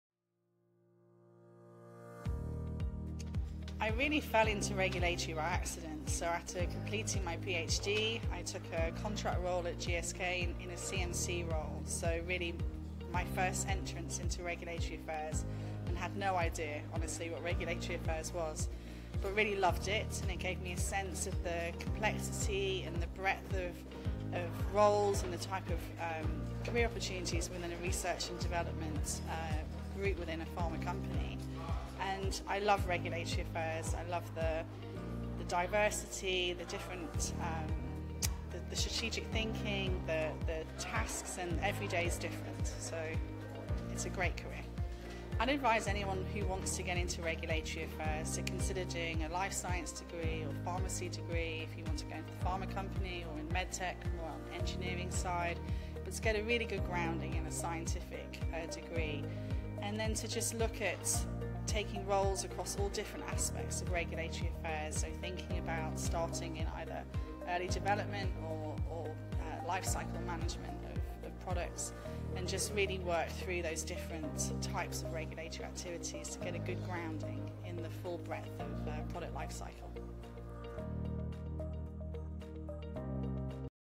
I am regulatory: Kate Stockman, GSK
Kate joined us at Euro Convergence 2022 to talk about the benefits of volunteering with RAPS, why she loves the regulatory profession and more.